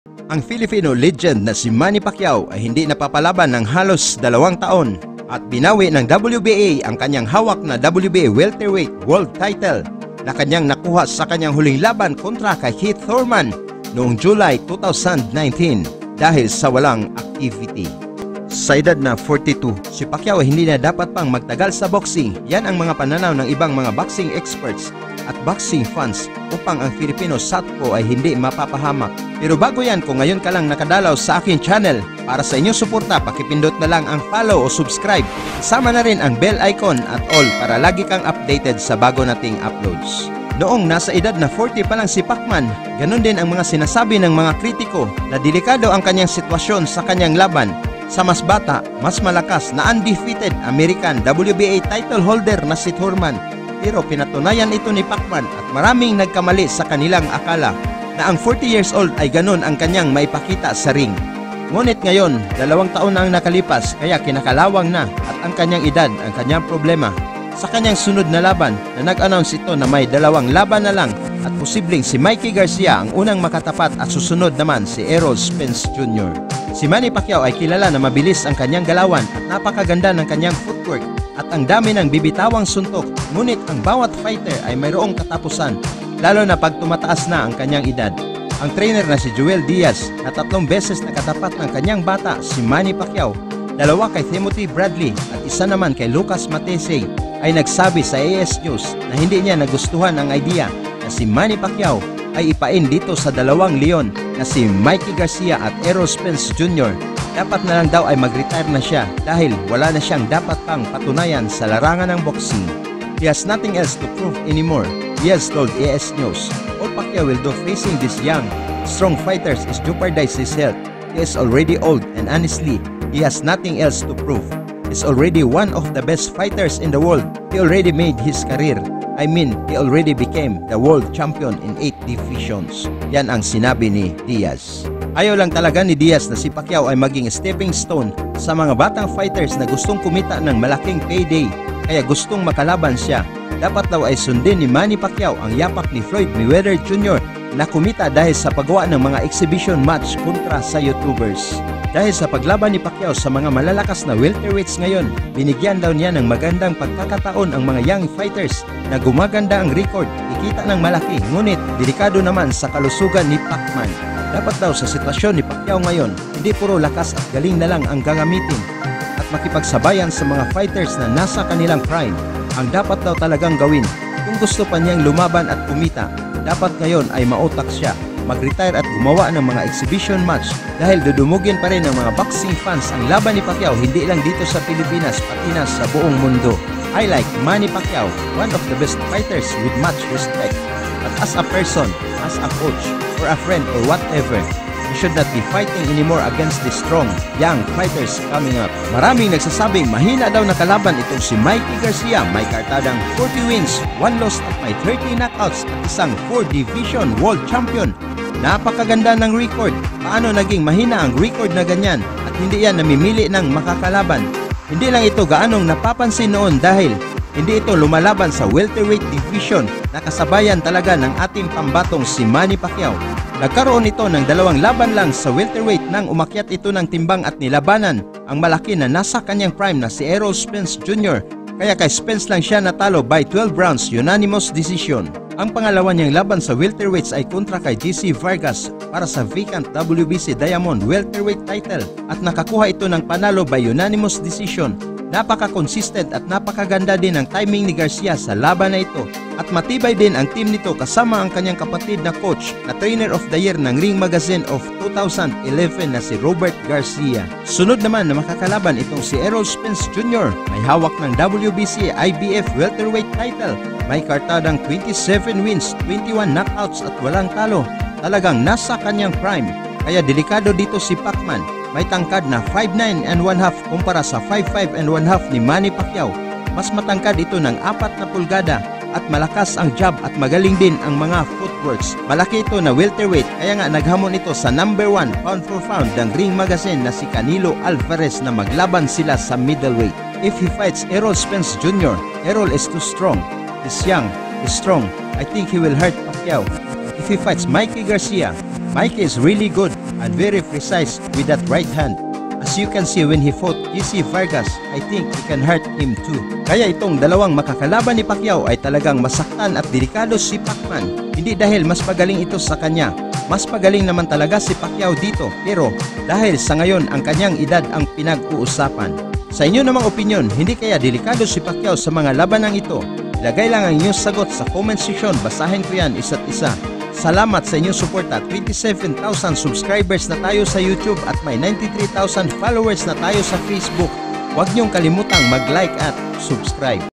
JOEL DIAZ: PACQUIAO MAG RETIRE KANA | GAYAHIN MO SI MAYWEATHER
MATANDA NA DAW SI PACQUIAO, KAYA WAG NA LUMABAN KAY MIKEY AT ERROL. DAPAT MAG RETIRE AT LUMABAN NG EXHIBITION FIGHTS, KATULAD NI MAYWEATHER